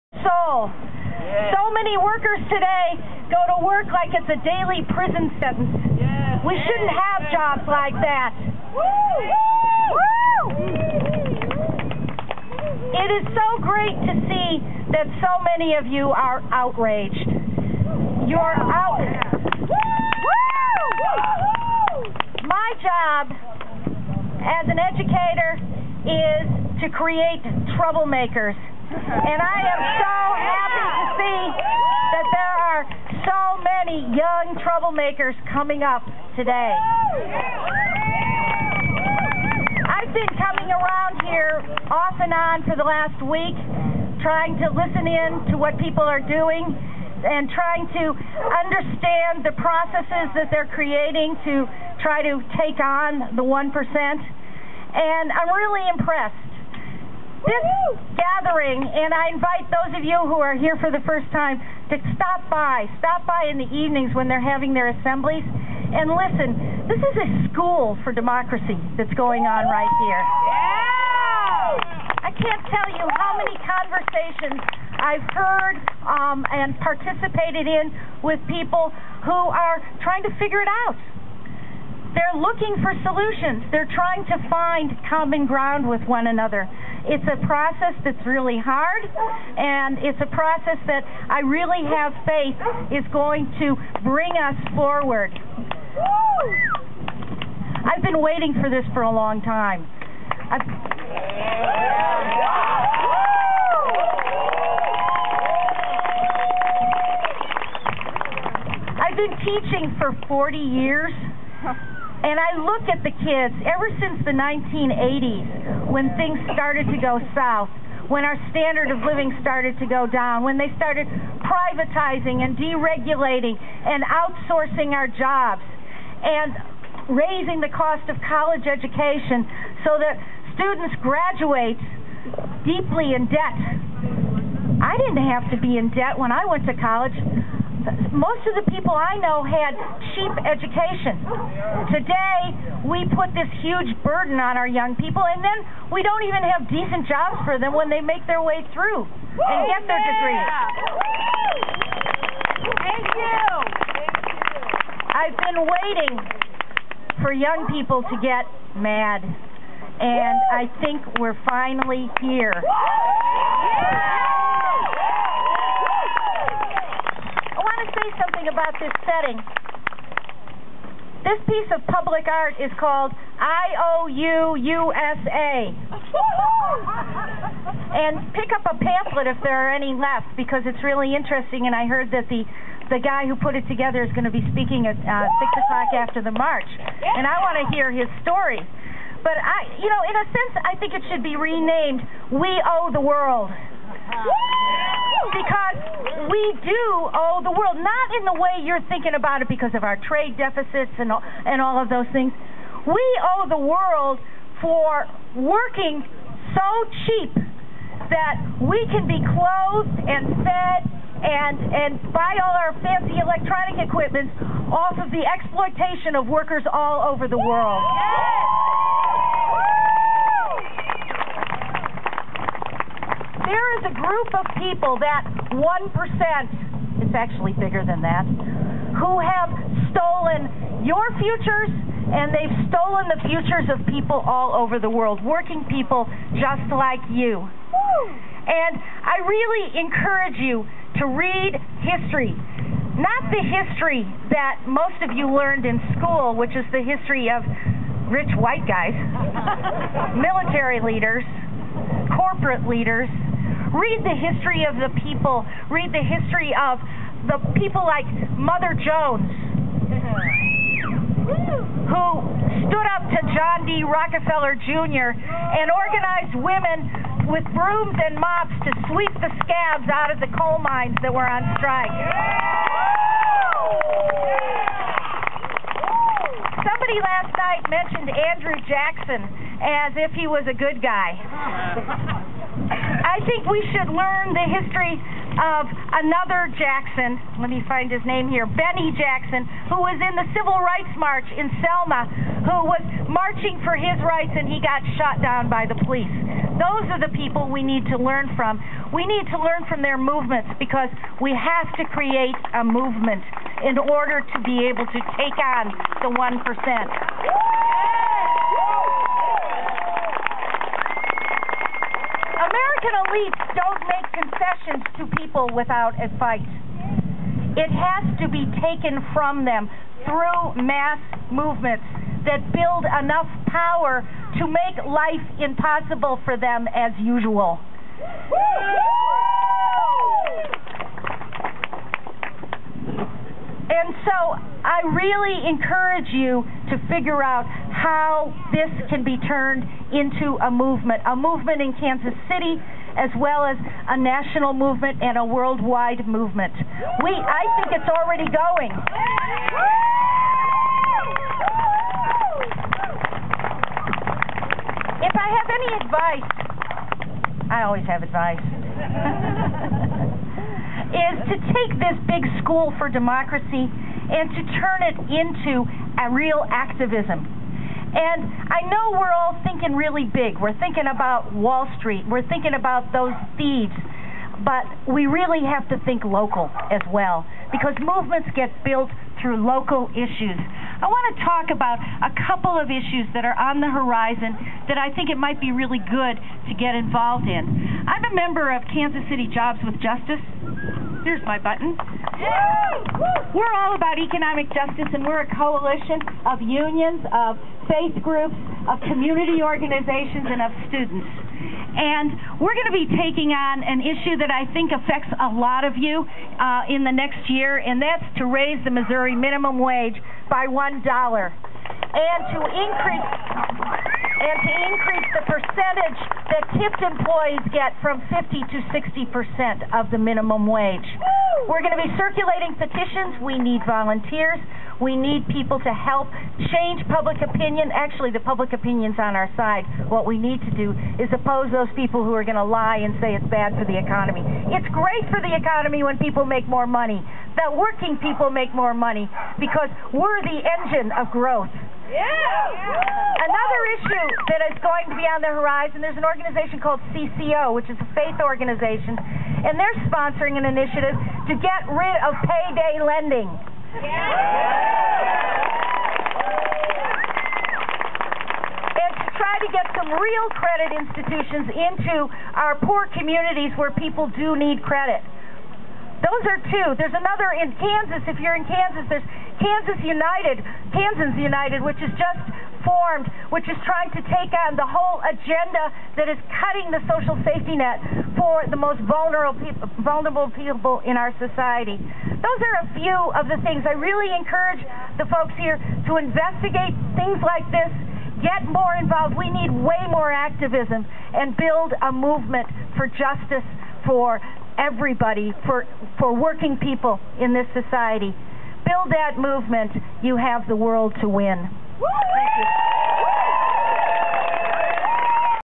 Judy Ancel Occupy KC 10-09-11.m4v
Judy Ancel's speech at Occupy KC rally on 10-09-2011 before the march. Sorry about the video I suggest you just listen to what she has to say and don't worry about watching. I was shooting with no tripod while trying to simultaneously eat my lunch. Sorry!